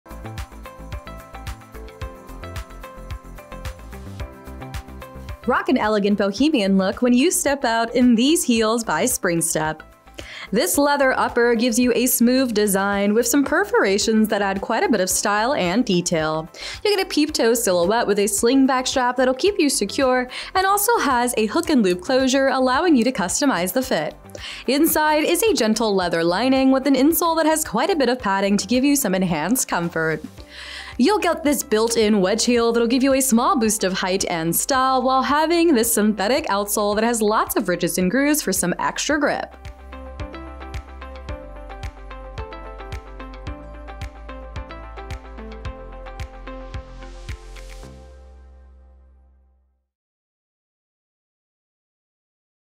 Spring Step Bohemianish SKU: 9850729
Exude an elegant and sophisticated vibe with every step you take wearing Spring Step® Bohemianish Heels.
Leather upper.
Cut-out detailing on upper.
Hook-and-loop closure.
Brand logo on insole.
Slight wedge heel.
Synthetic outsole.
Imported.

Product measurements were taken using size EU 40 (US Women's 9), width M. Please note that measurements may vary by size.